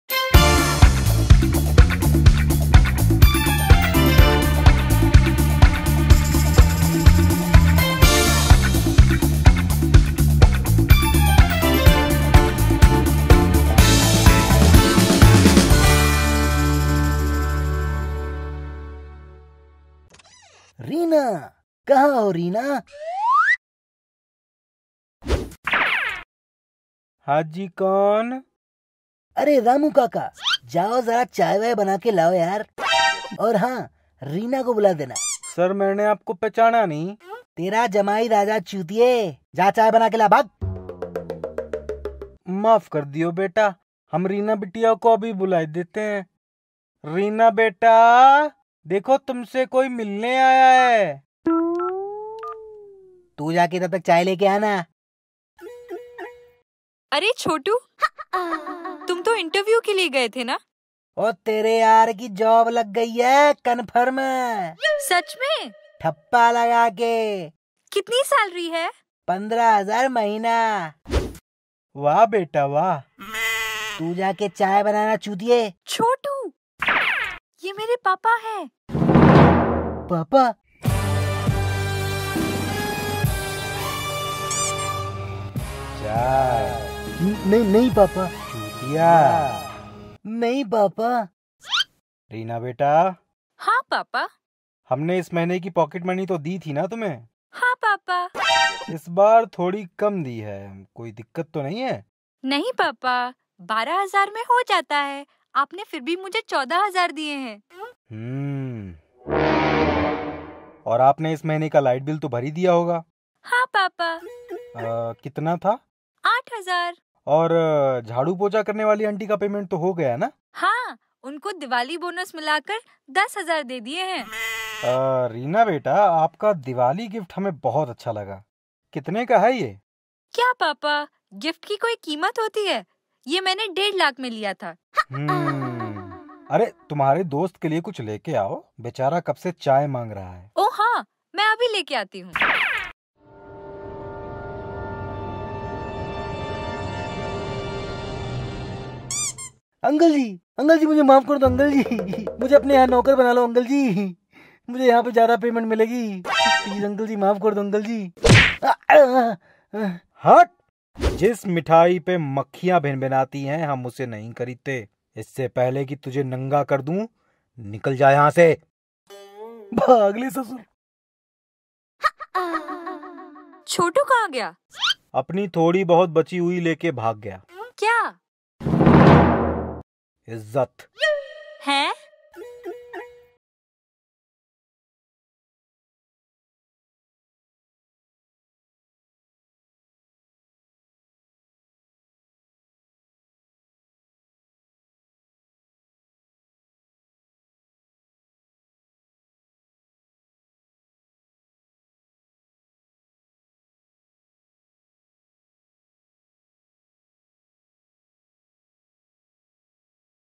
Chotu ki Girlfriend | छोटू की गर्लफ्रेंड | Hindi Comedy Cartoon Story | Goofy Works
Chotu ki Girlfriend | छोटू की गर्लफ्रेंड
Watch Chotu facing consequences with his girlfriends dad after meeting.

Video Credits,
Animation & Direction: Sachin Chauhan
Story Screenplay: Sachin Chauhan
Voice Artists: Sachin Chauhan, Khushi Chauhan